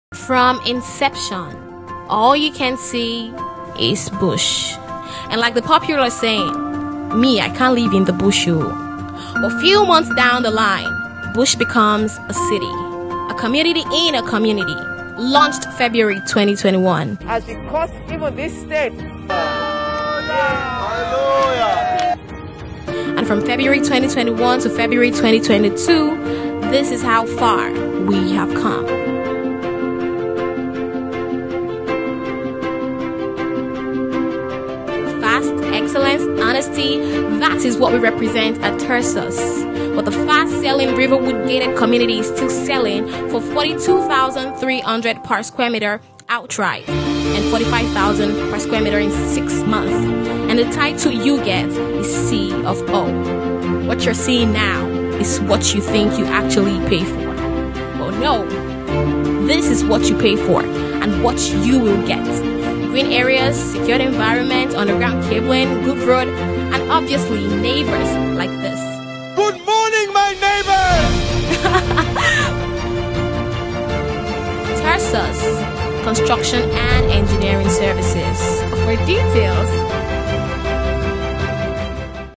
Riverwood Gated Community Estate Land With C of O For Sale #08069017662
RIVERWOOD GATED COMMUNITY
Loocated at Kilometer 35, Lakowe Ibeju lekki.

An undeveloped
land, which is currently being developed by us

It's an Ideal location to live and invest in. It is  5 mins away from the Lakowe Golf & Country Estate.

Plot Size: 300sqm upwards

Price: N42,300 Per sqm

FACILITIES INCLUDE;
PAVED ROAD
LANDSCAPING
RECREATIONAL FACILITIES
UNDERGROUND DRAINS
SECURITY

Total cost of land is inclusive of the following;
Legal Fees
Cost of Sand filling
Survey
Development Levy - which covers;  Walkway Road, finished in paving stone,
Perimeter fence,
Gate house,
Facility manger office,
Green area,
Underground drainage for storm water,
Piping for central water,
Underground piping for electrical cables,
Landscaping.

Buyers will obtain:
Deed of assignment from the global C of O covering the land, letter of allocation and survey of land